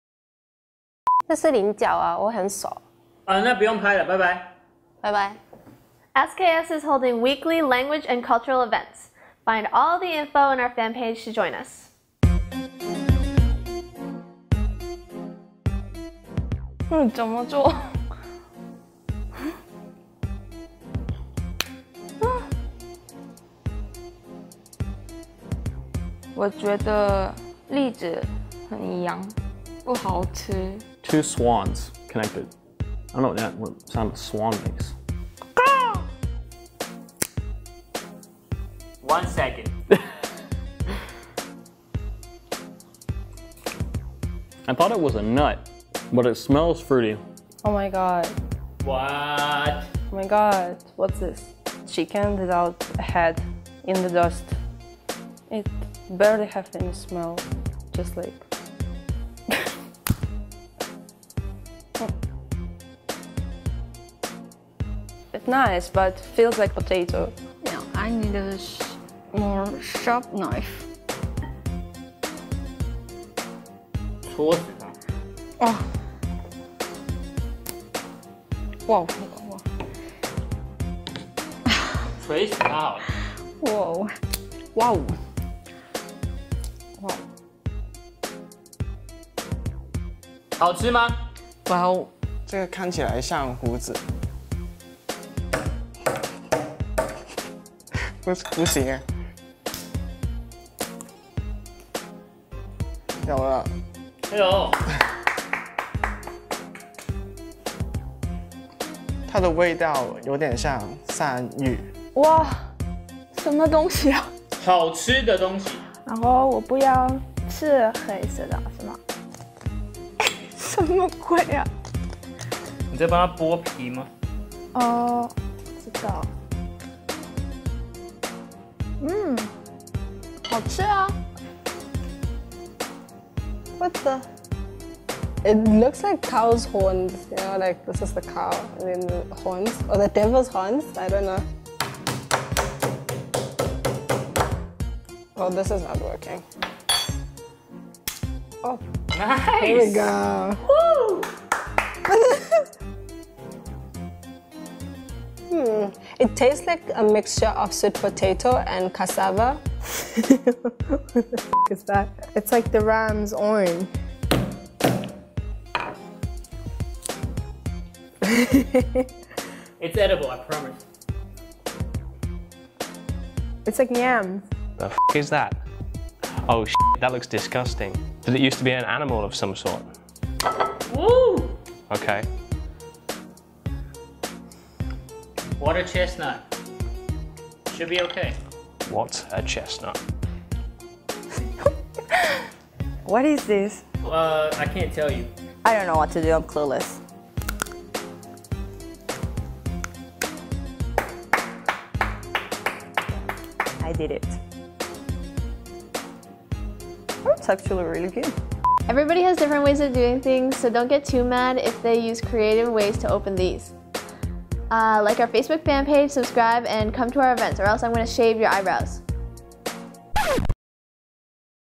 讓老外束手無策的台灣特產 ft.菱角: Foreigners Can't Open Taiwanese Water Chestnut?
贊助商: 微克詩國際事業有限公司